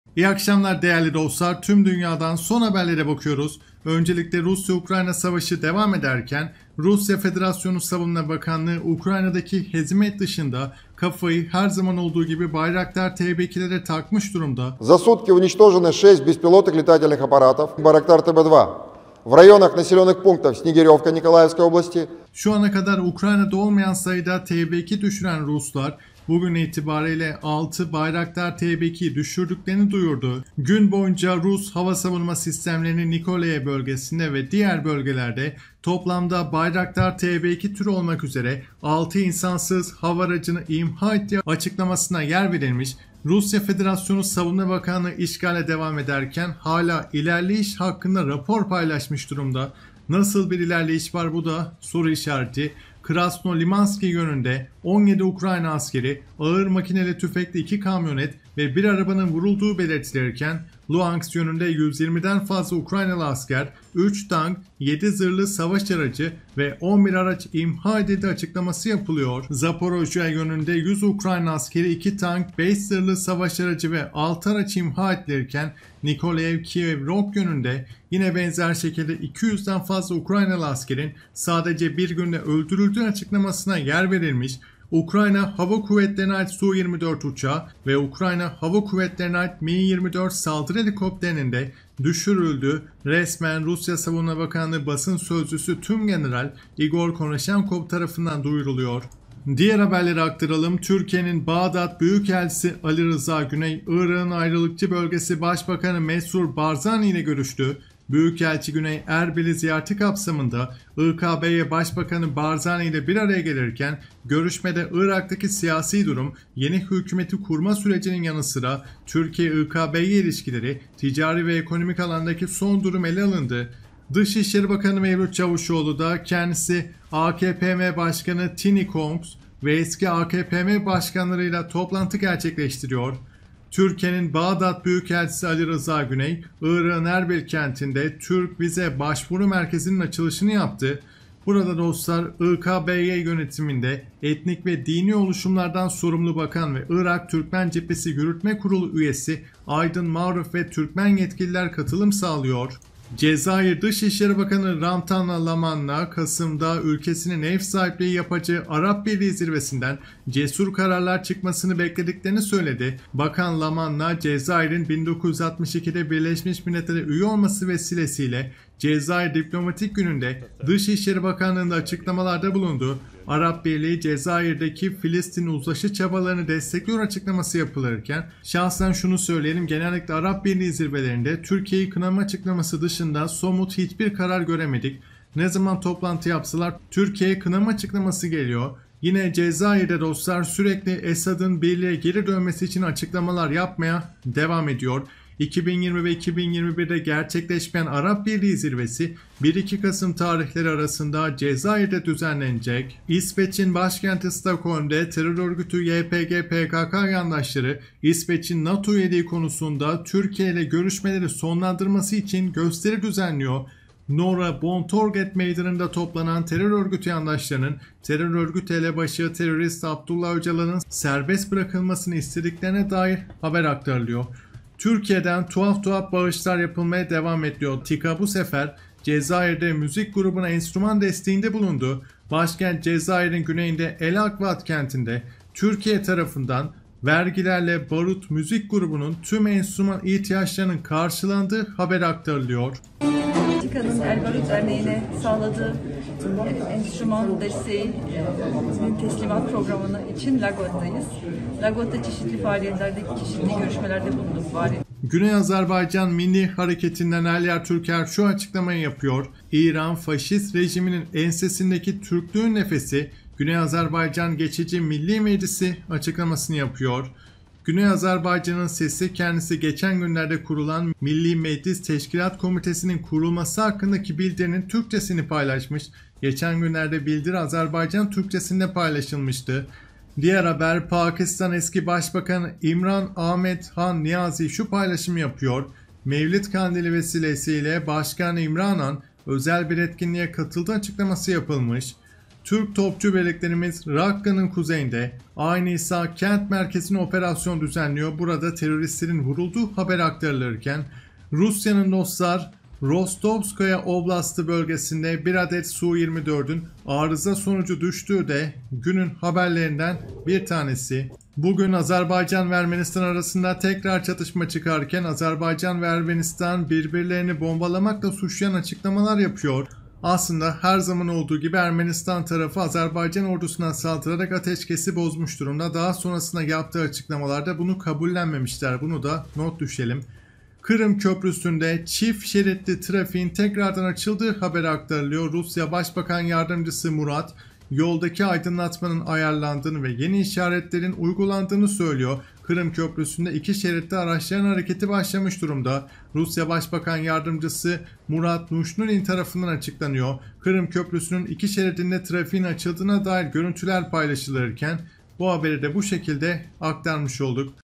RUSYA: 1 GÜNDE 6 BAYRAKTAR TB-2 DÜŞÜRDÜK ! RUS YALANI : İLERLİYORUZ ! UKRAYNA KIRIM KÖPRÜSÜ ONARILDI
Crimean bridge partially collapsed after explosions. Fuel containers can be seen on fire.
Yarımadayı Rusya anakarasına bağlayan Kırım Köprüsü'nde meydana gelen patlamada ciddi hasar meydana geldi. Köprünün bazı bölümleri suya düştü. Bu köprü, Putin hükümetinin amblemlerinden biridir. Bunlar Ruslar için şok edici görüntüler.
Rusya'ya bağlı RIA haber ajansı, Cumartesi günü erken saatlerde Kırım'daki Kerç Köprüsünde bir yakıt tankının infilak ettiğini duyurdu. Ukrayna kaynakları, haberi patlama olarak geçti.

RIA, yerel bir yetkiliye dayandırdığı haberinde, "Ön bilgilere göre, Kırım köprüsünün bölümlerinden birinde bir yakıt deposu yanıyor, nakliye kemerleri hasarlı değil" ifadelerine yer verd
rusya'nın devlete bağlı RIA haber ajansı, Kırım'daki Kerç köprüsünde bir yakıt tankerinin patlayarak yanmaya başladığını bildirdi. Köprü Rusya'nın Kırım'ı 2014'te ilhak ettiği süreçte büyük stratejik önem taşımıştı.
Yerel yetkililere dayandırılan haberlerde "Ön bilgilere göre, Kırım köprüsünün bölümlerinden birinde bir yakıt tankı yanıyor" denirken nakliye kemerlerinde hasar olmadığı bildirildi.

Köprüde Trafik durduruldu.

Ukrayna medyası, köprüde sabah saat 06.00 sıralarında bir patlama meydana geldiğini bildirdi.